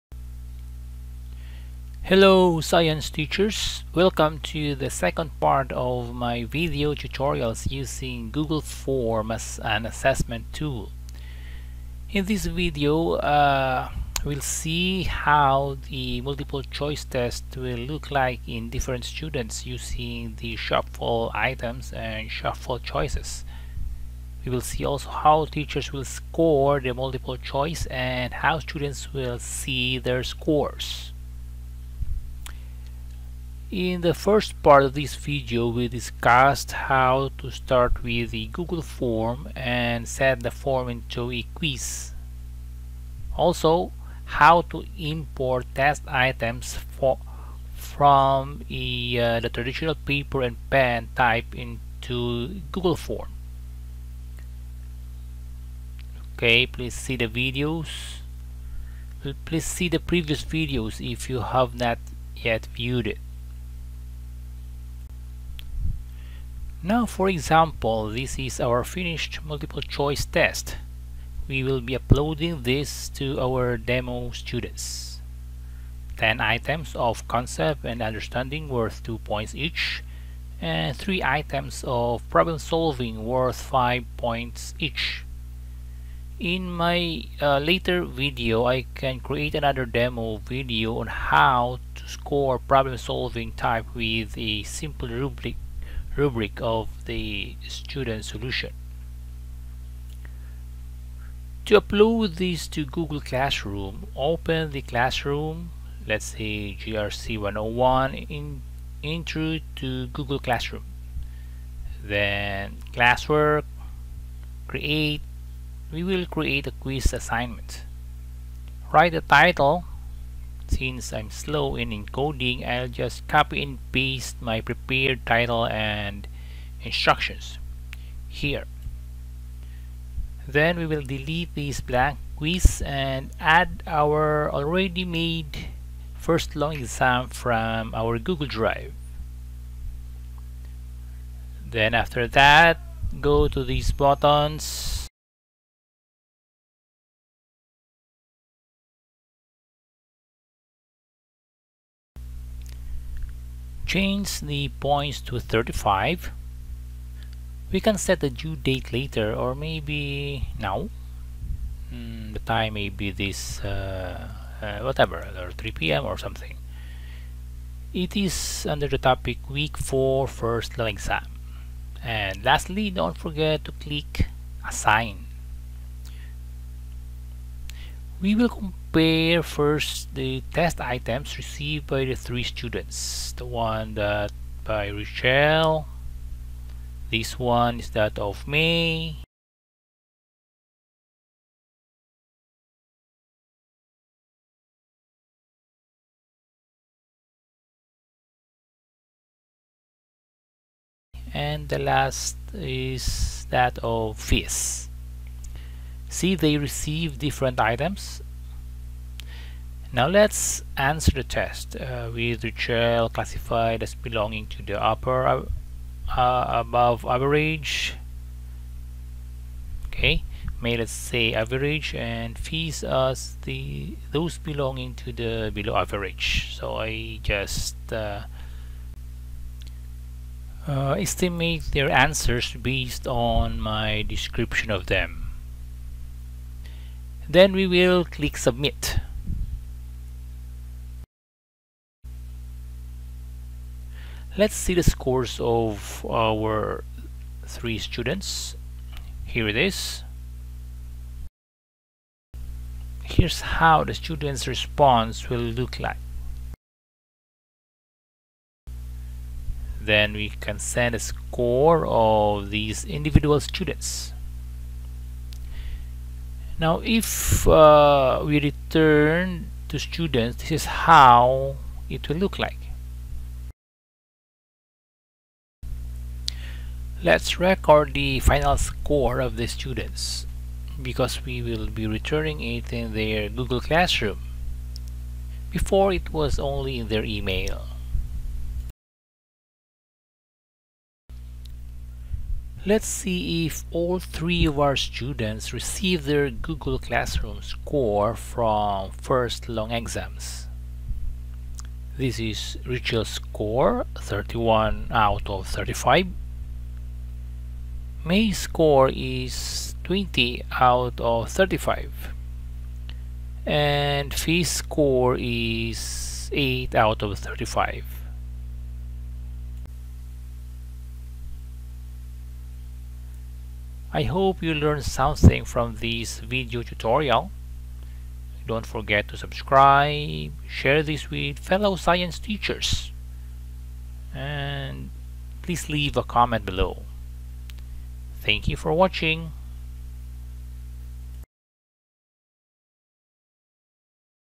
Google Form as an Assessment Tool (Part 2)
This is Part 2 of the video demonstrating how a Google Form can be used as an effective Assessment Tool. Shown in this video were steps on how to upload a prepared Google Form into the Google Classroom. Teachers can determine the score of the students right away and can return the scores immediately to the students for immediate feedback.